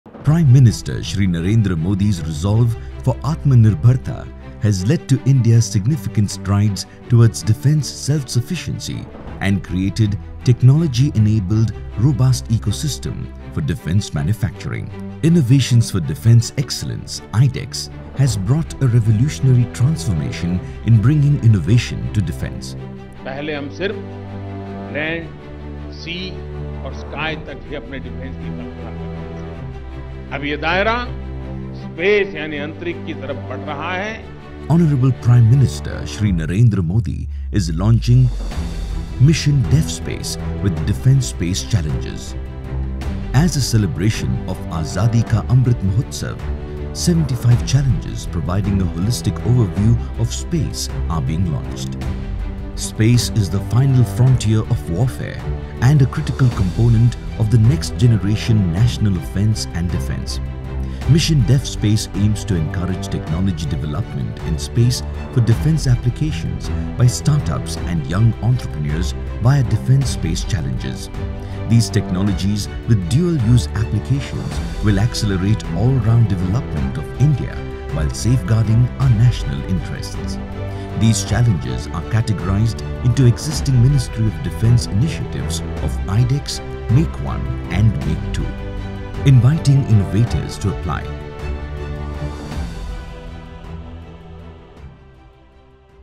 Mission DefSpace - Developing innovative solutions for the Defence Forces in space!
Prime Minister Narendra Modi launched Mission DefSpace -  to develop innovative solutions for the Defence Forces in the Space domain through industry & startups.